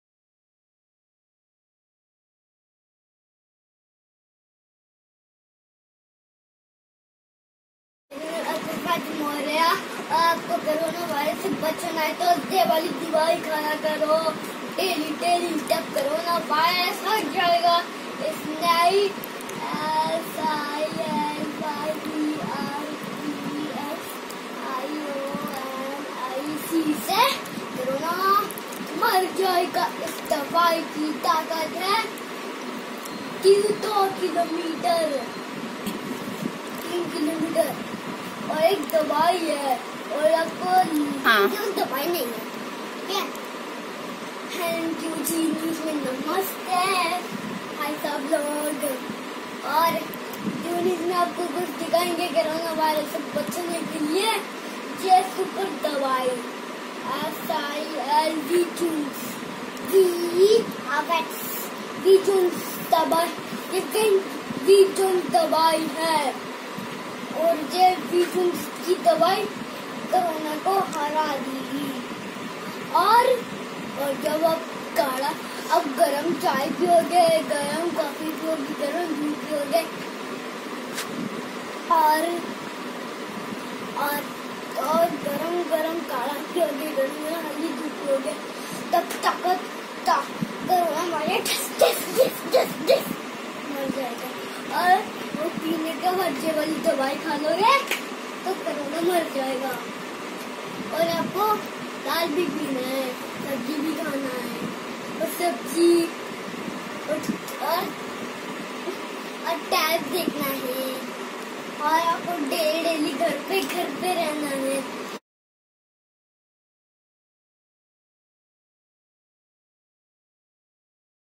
funny video for kids/funny video made by kid on corona
This is funny video and only made for fun. In this Video kid is talking and it is not related to any thing. It is not scripted , kid is speaking on his own.
Kid is doing a role play act. This kind of activity gives them a confidence to speak on topics.